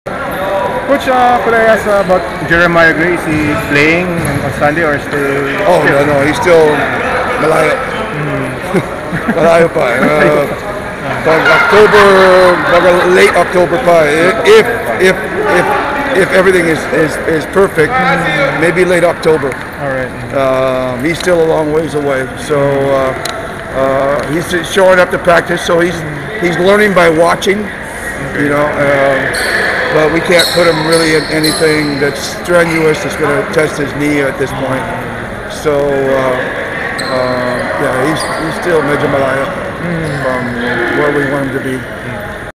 Ginebra targets late October debut for Jeremiah Gray | PBA Commissioner's Cup
Tim Cone on when Jeremiah Gray will play: "Malayo pa"

Interview: September 18, 2022